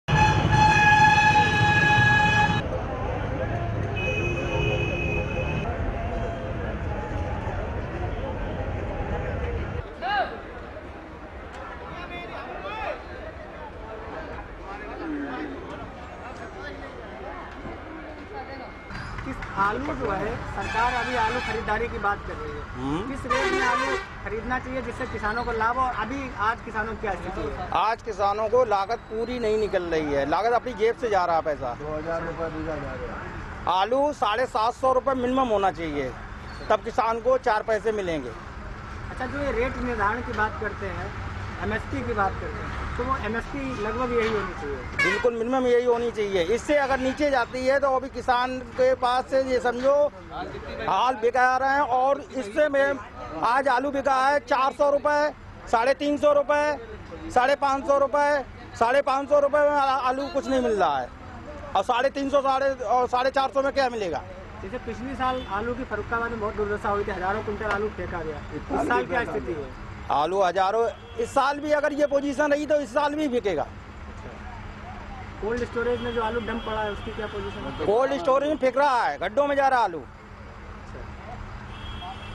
फर्रुखाबाद: आलू की फसल का नहीं मिल रहा उचित दाम, परेशान किसान
फर्रुखाबाद : आलू की खेती ने इस बार फिर किसानों को रोने के लिए मजबूर कर दिया है. अभी पुराने आलू के घाटे से किसान उबर भी नहीं पाये थे कि मंडियों में नए आलू भी उचित कीमतों पर नहीं बिक रहे हैं.  इससे किसानों को मुनाफा तो दूर उनकी लागत तक नहीं निकल पा रही है.